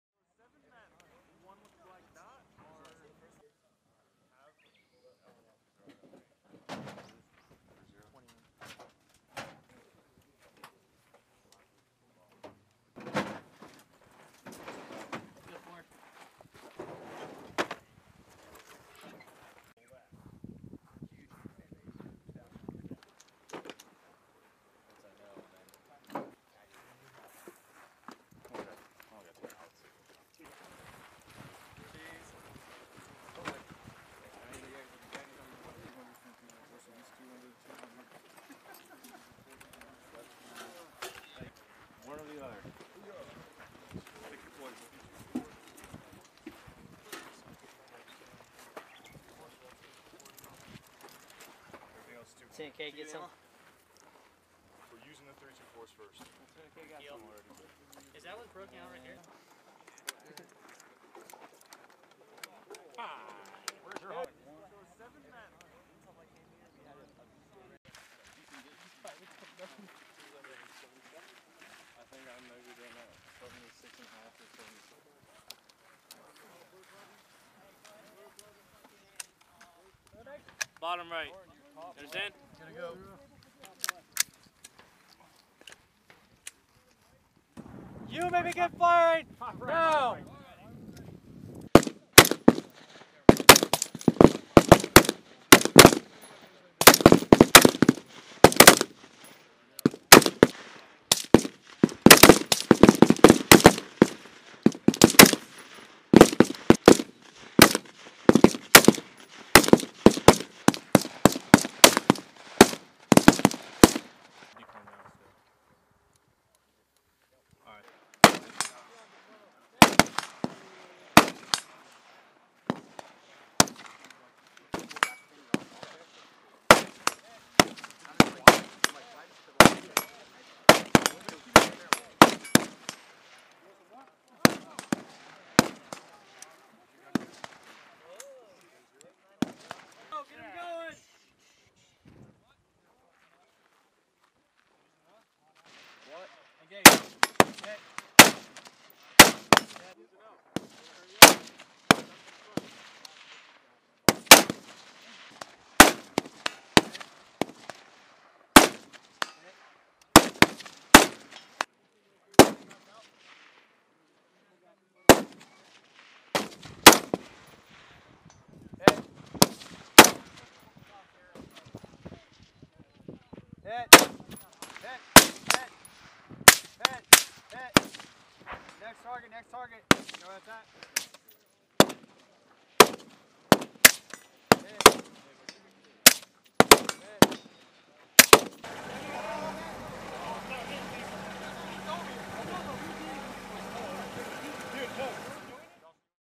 US Marines with 1st Battalion, 6th Marine Regiment conduct unknown distance range during Fuji Viper
U.S. Marines with 1st Battalion, 6th Marine Regiment, currently assigned to 3d Marine Division conduct an unknown distance rifle range during exercise Fuji Viper 20.4 on Camp Fuji, Japan, June 17. 2020. Fuji Viper is a regularly scheduled training exercise that allows Marines to maintain their proficiency in infantry and combined arms tactics. 3d Marine Division remains ready to respond to any crisis or contingency in the Indo-Pacific region.